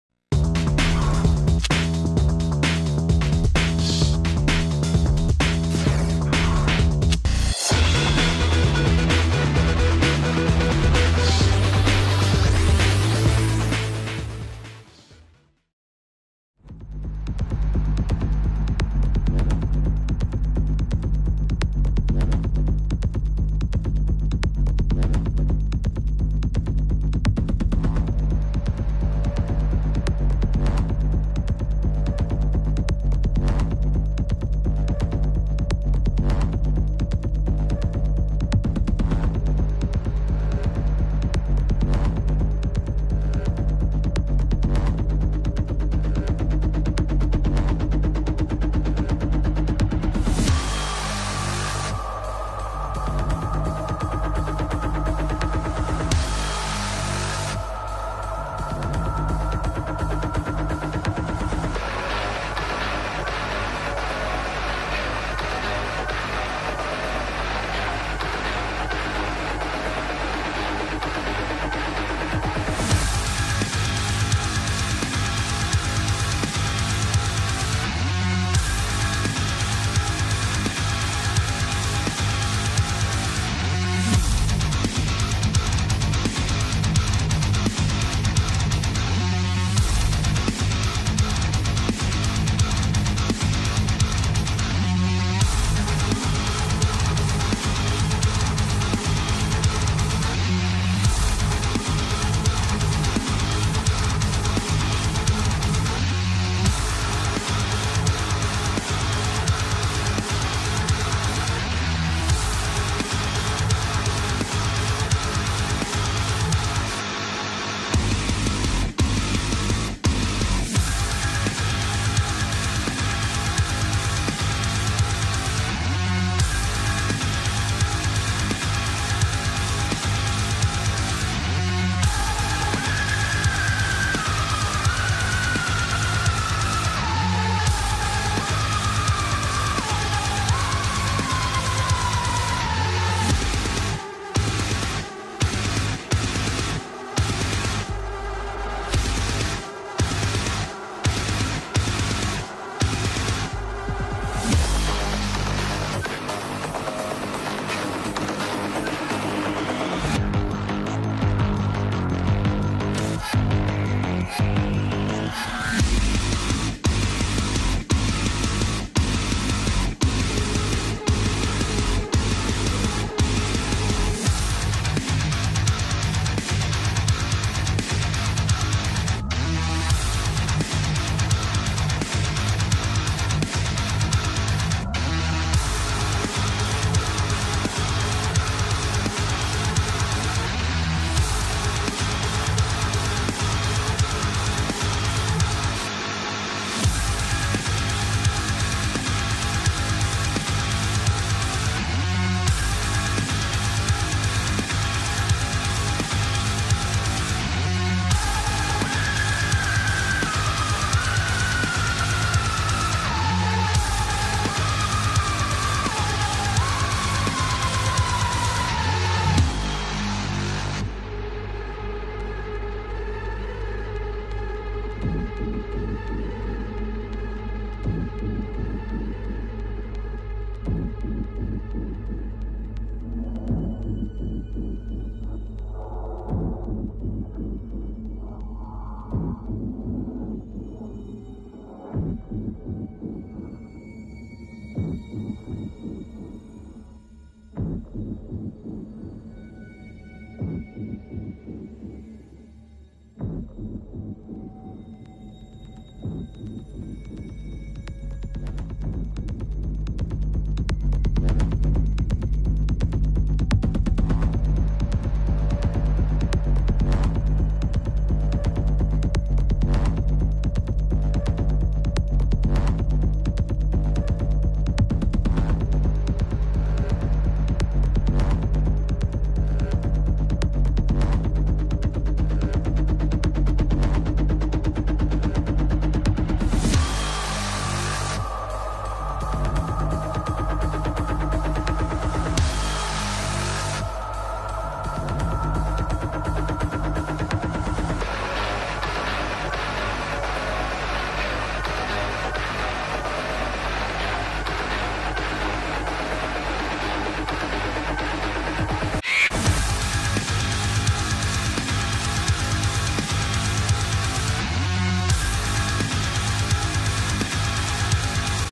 Drawing a Rocker Bird and Made a Rock Cover Timelapses 🎸 - SkeletoonsWasHere
I remake a rock and roll bird and make a rock cover